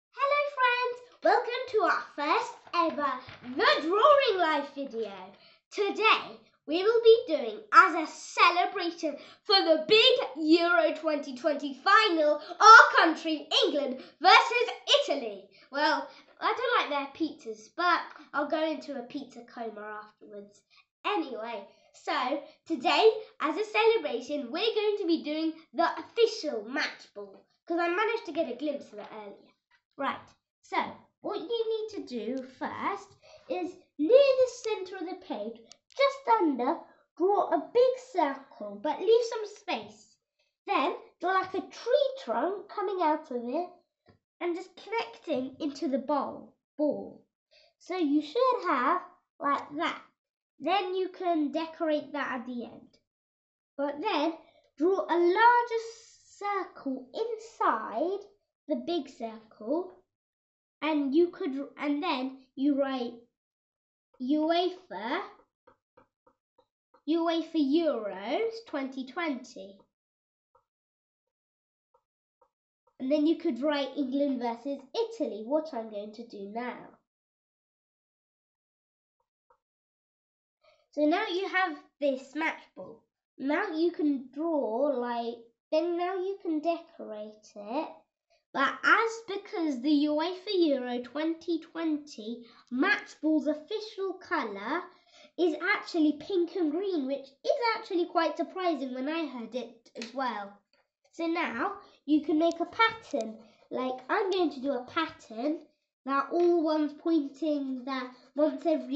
Part 1 - How to draw the official EUFA Euro 2020 Match ball
I know all the world has been waiting for this moment. Welcome to the EUFA football drawing club!! Part 1 and Part 2 later today!!!